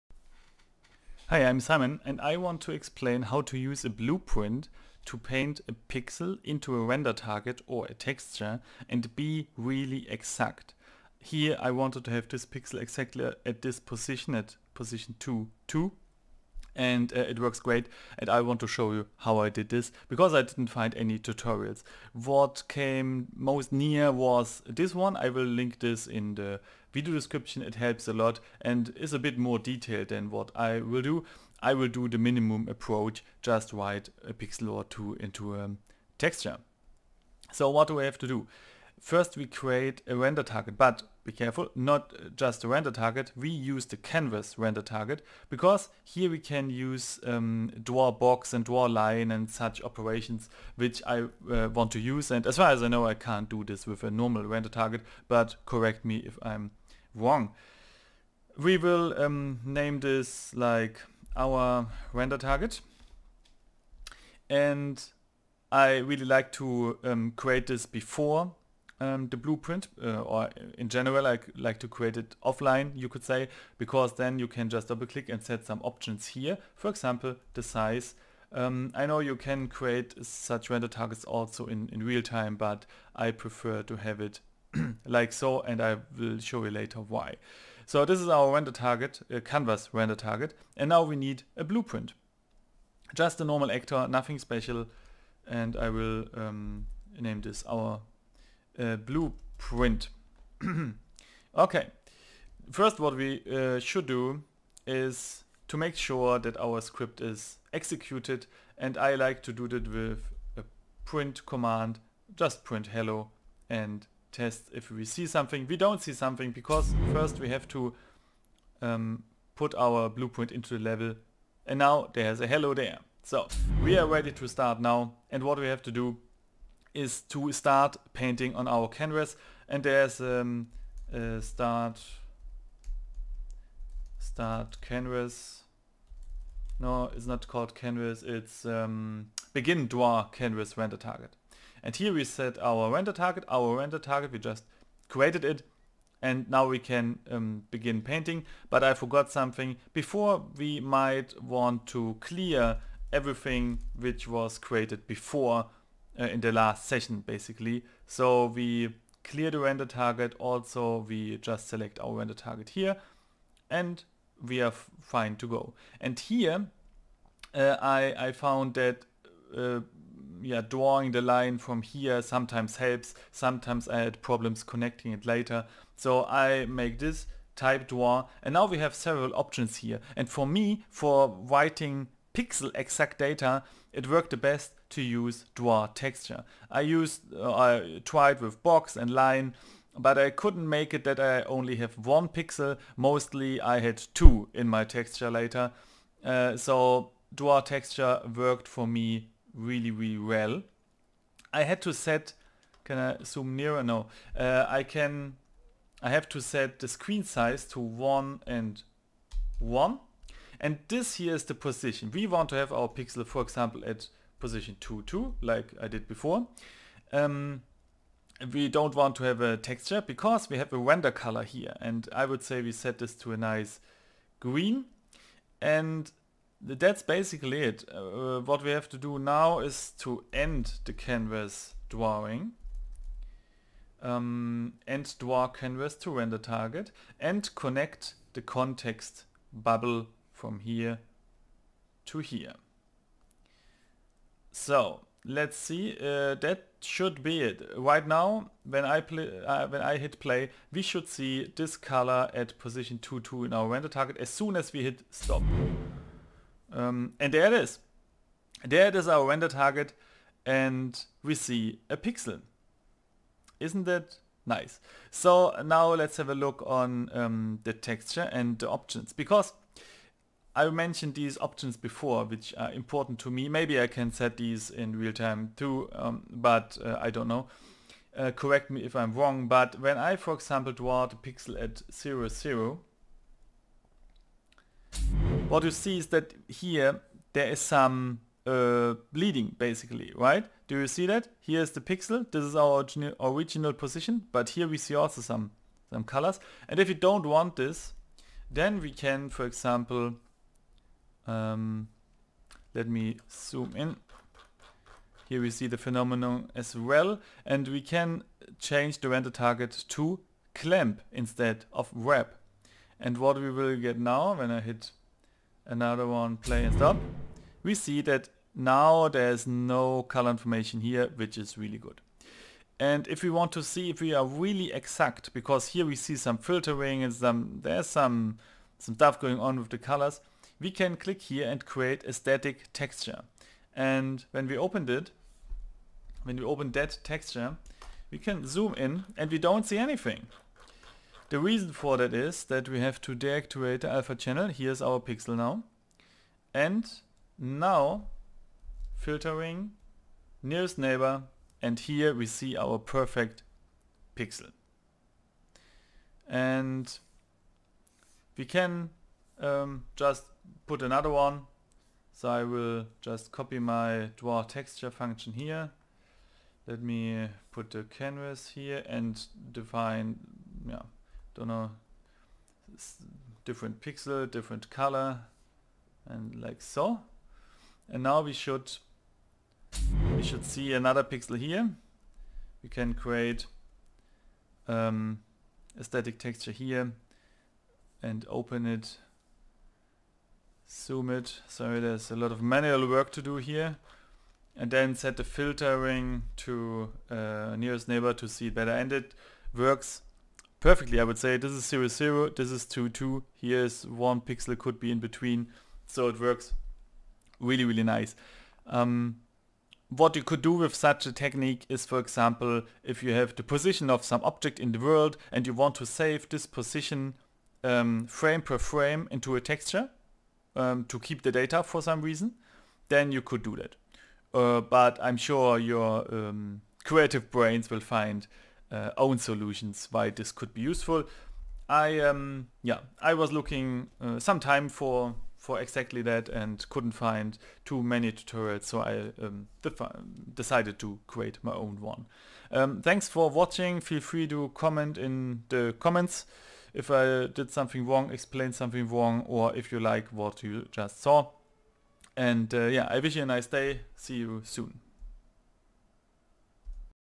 Write Pixel via Blueprint to exact positions in Canvas RenderTarget/Texture (Unreal Engine 4.14)
I want to write pixel per pixel into a texture / renderTarget and show how to do that by using Canvas Render Target and a Blueprint